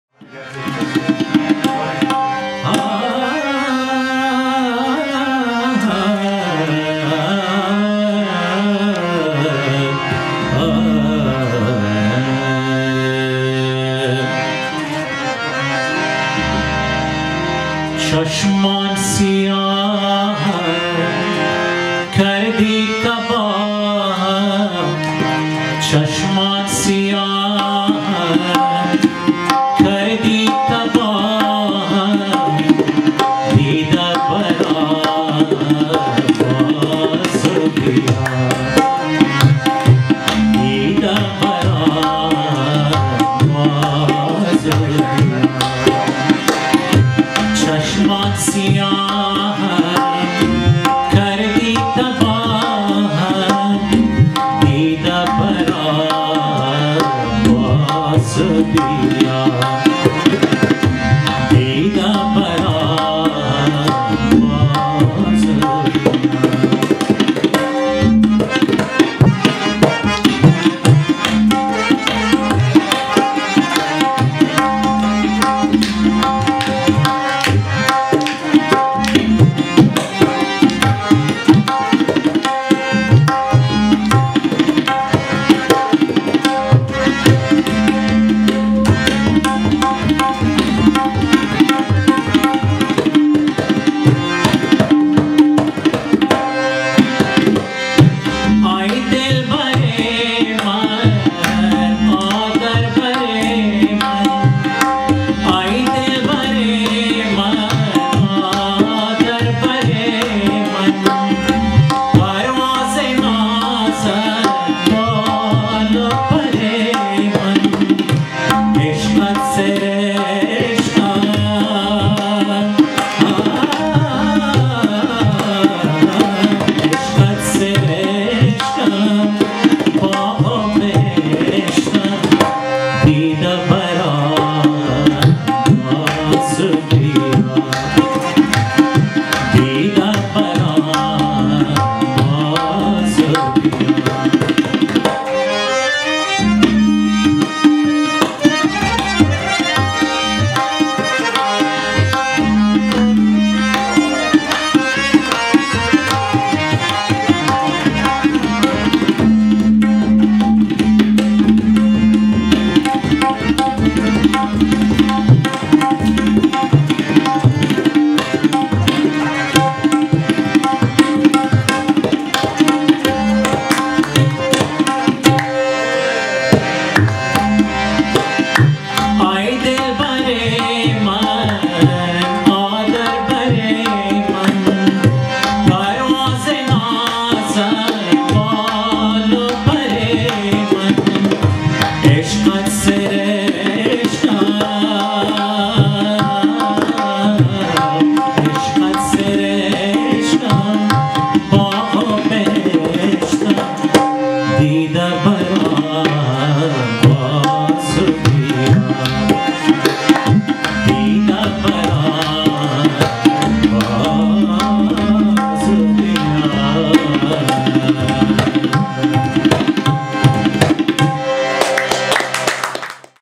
Said Daoud live
performing live in VA with Sayed Masood on tabla Sept. 2014